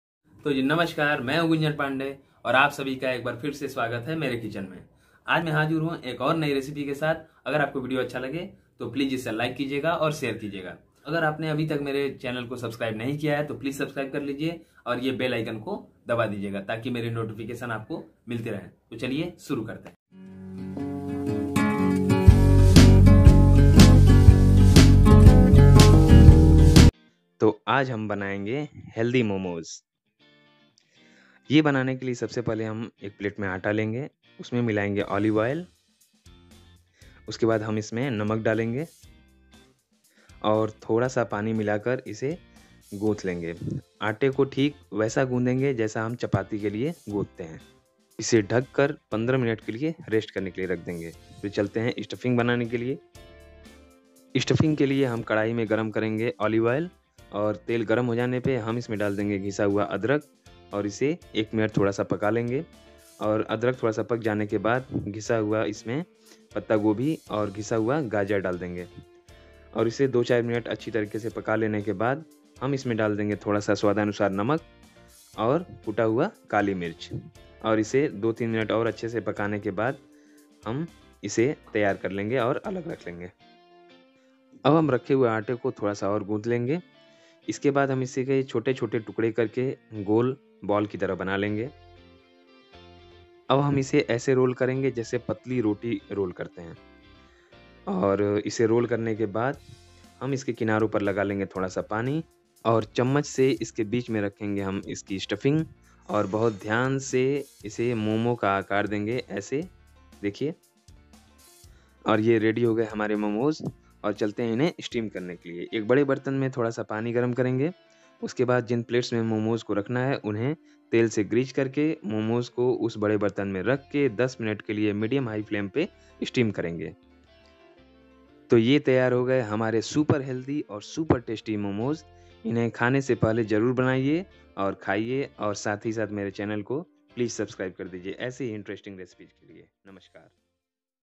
Veg Momos Recipe | मोमोज बनाने की विधि | Veg Dim Sum | adarshkitchen
In this healthy veg momos recipe I have shown the easy steps to make healthy veg momo. Veg momos is delicacy of Nepal, Veg momo is very popular in India and is considered one of the delicacies from east Indian cuisine. This Healthy Veg Momos Recipe by adarshkitchen is extremely easy to follow. Momos is veg dumplings which called Veg Dim Sum in Nepal.

Ingredients-
Whole wheat flour- 250 gm
Sesame or olive oil- 2 tbsp
Rock salt- 1 tsp
Olive oil- 2 tbsp
Grated ginger- 1 tbsp
Grated cabbage- 75 gm
Grated carrot- 50 gm